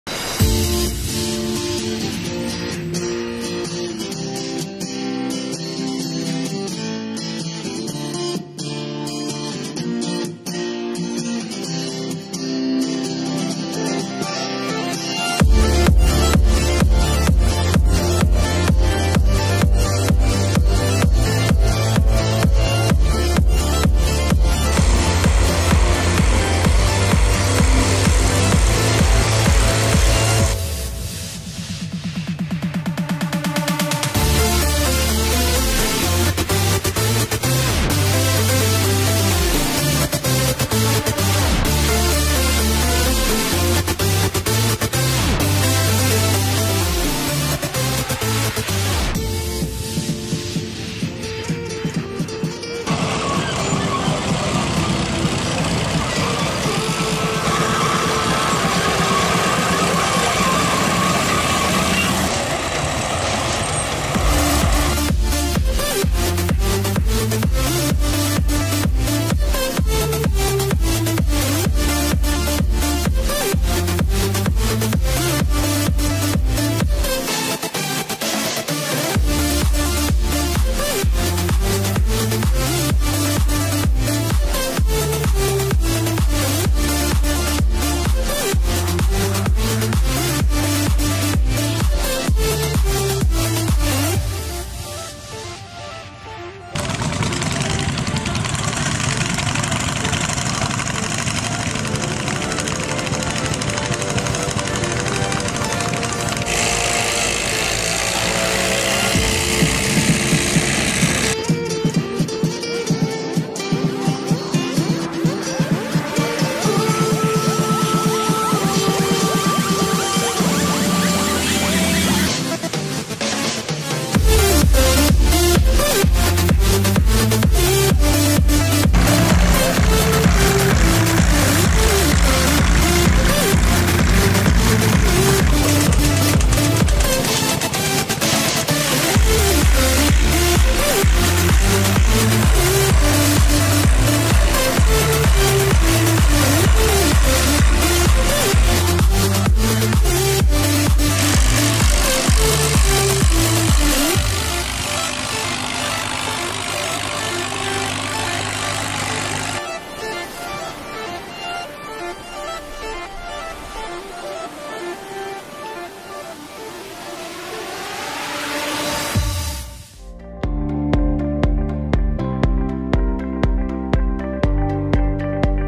Festa dell'Aratura Quarantoli | Ploughing Party | Fiat - Caterpillar - Landini
IT: Come ogni anno, ad Agosto, si svolge la festa dell'Aratura, evento nel quale, in collaborazione con vari collezionisti di trattori d'epoca, è possibile vedere mezzi restaurati completamente funzionanti lavorare come un tempo venivano usati quotidianamente. I mezzi usati sono vari, a partire dai Landini testa-calda, a Fiat fino a Caterpillar.
Si ringraziano tutti i partecipanti e organizzatori.

EN: Like every year, in August, there is the Ploughing Party in Quarantoli; where in collaboration with some collectors of old tractors, it's possible to see old restored tractors at work as it used to be every days in the past. The tactors at work were: Landini , Fiat , Caterpillar, ecc.
Thanks to all the organizers of this event.